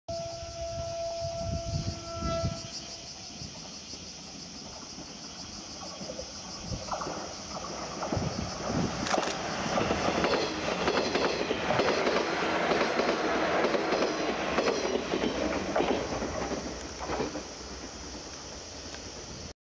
伊豆箱根鉄道を走る185系電車の｢踊り子｣
伊豆箱根鉄道を走る185系電車の｢踊り子｣です。